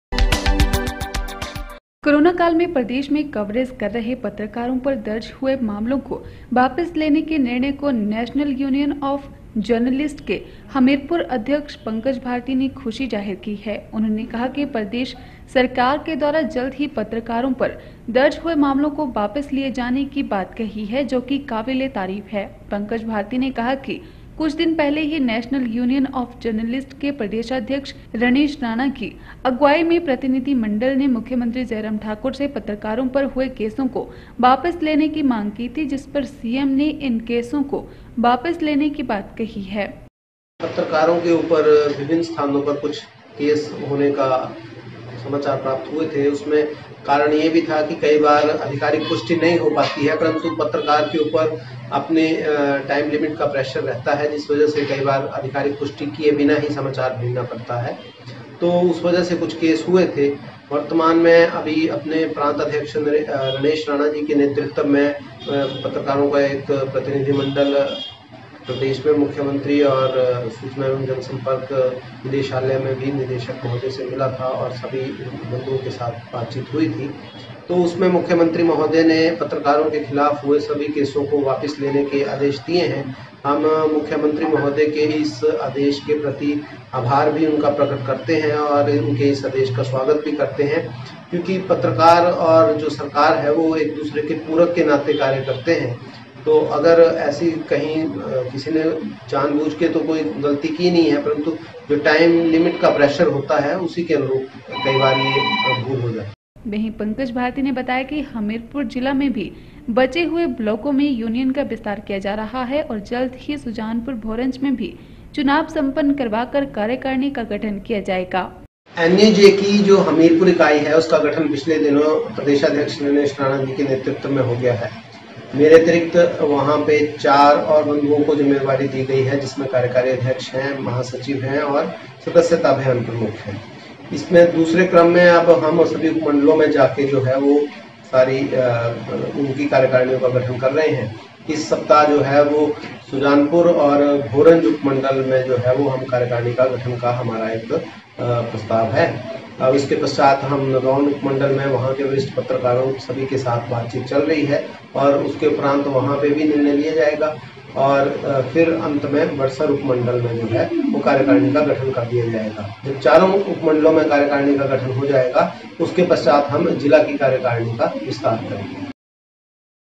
7 july 2 पत्रकारों पर दर्ज हुए मामलों को वापिस लिए जाने की बात काबिले तारीफ: अध्यक्ष पंकज भारती
कोरोना काल में प्रदेश में कवरेज कर रहे पत्रकारों पर दर्ज हुए मामलों को वापिस लेने के निर्णय को नेशनल यूनियन आफ जर्नलिस्ट के हमीरपुर अध्यक्ष पंकज भारती ने खुशी जाहिर की है। उन्होंने कहा कि प्रदेश सरकार के द्वारा जल्द ही पत्रकारों पर दर्ज हुए मामलों को वापिस लिए जाने की बात कही है जो कि काबिलेतारीफ है।